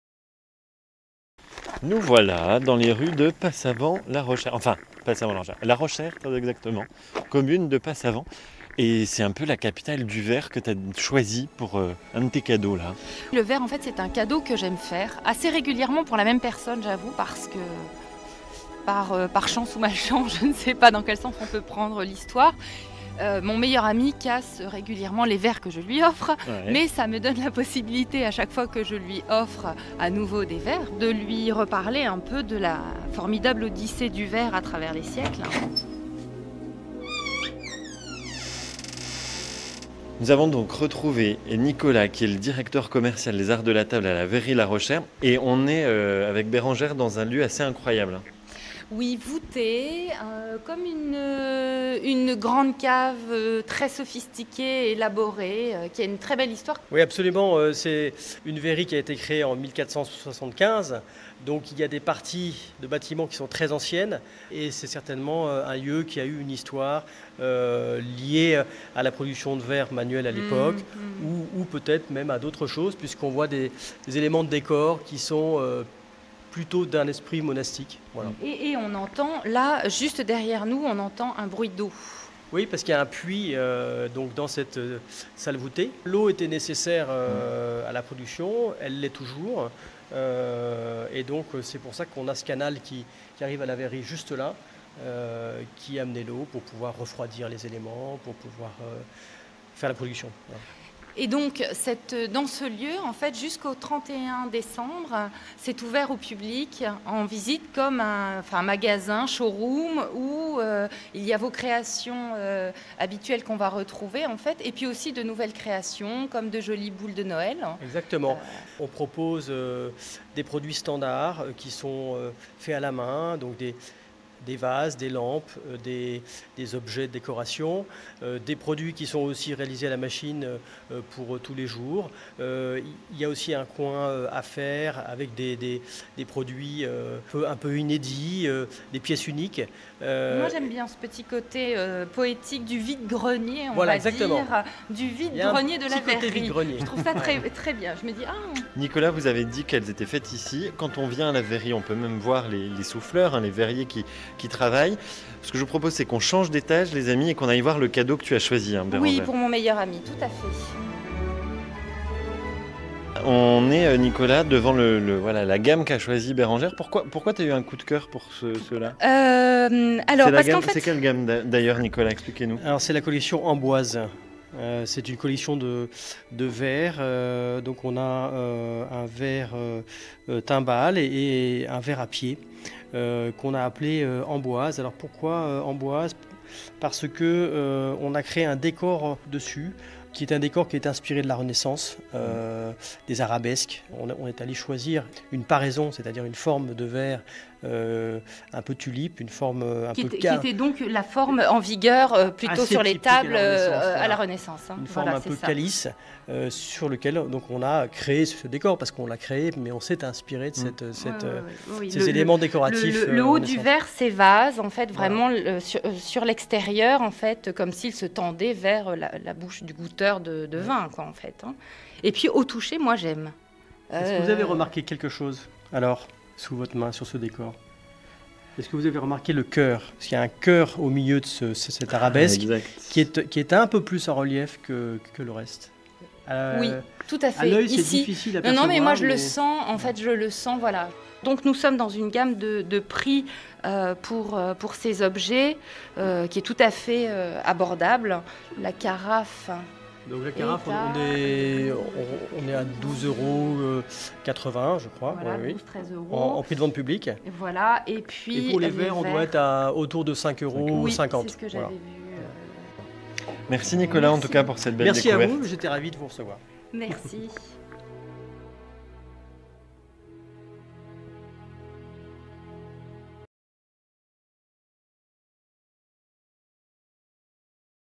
Carnet de Noël en Franche-Comté : à la Verrerie de la Rochère
Bérengère Desmettre, blogueuse, vous fait découvrir la Franche-Comté autrement.